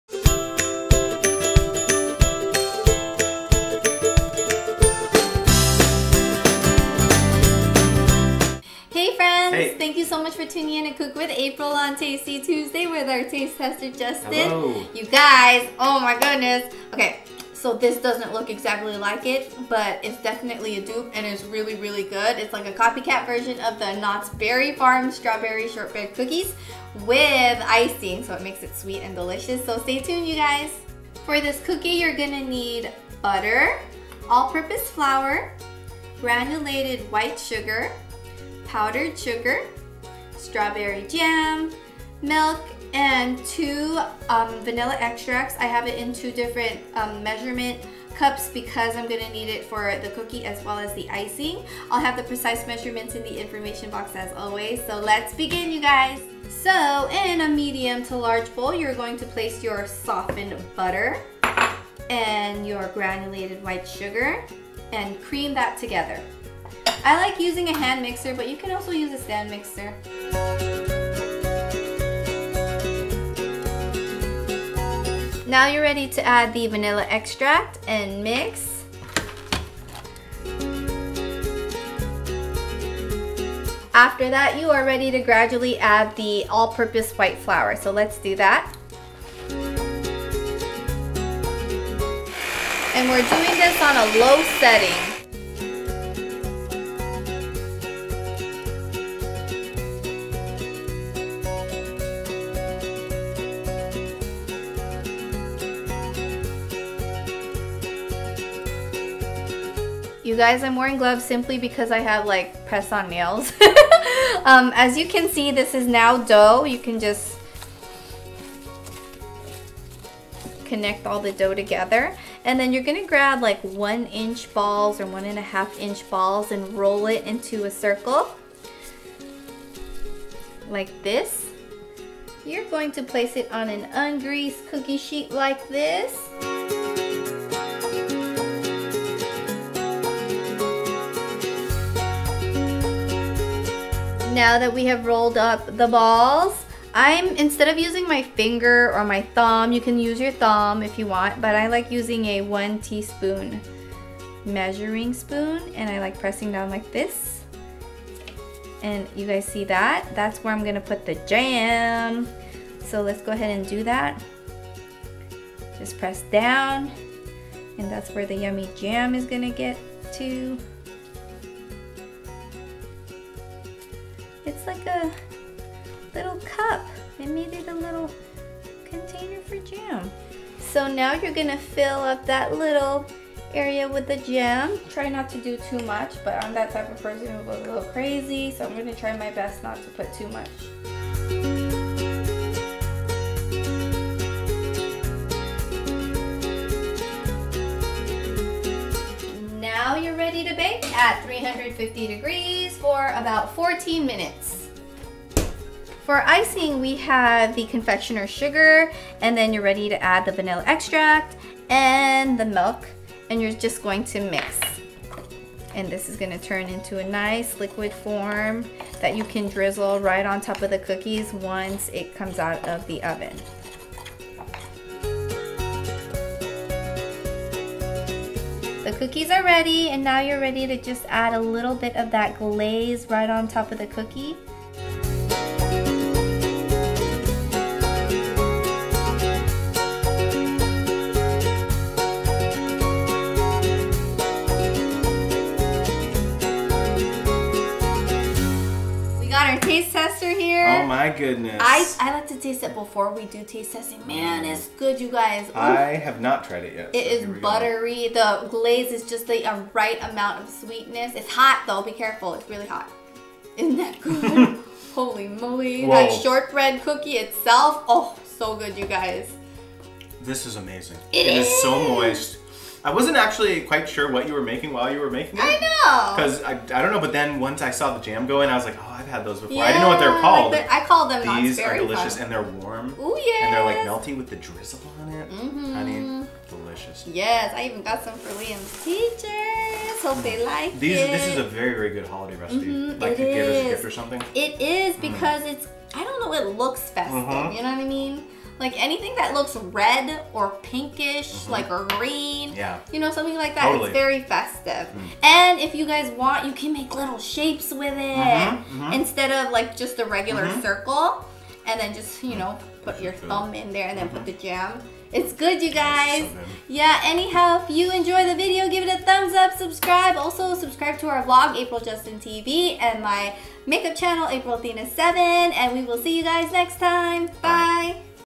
KNOTTS BERRY FARM STRAWBERRY SHORTBREAD COOKIES
Happy Tasty Tuesday!  Today I'm going to show you how to make these amazing copycat Knotts Berry Farm Shortbread cookies!  I've always wondered how to make these shortbread cookies and they are so easy! This is a perfect last minute Christmas gift as well! Enjoy!

✿ Ingredients:
1 cup butter
1 cup sugar
2 cups flour
1/2 cup strawberry jam
1/2 cup powdered sugar
1 tsp vanilla extract
1/2 tsp vanilla extract (separate)
1 tsp milk

Bake at 350F for 14 minutes.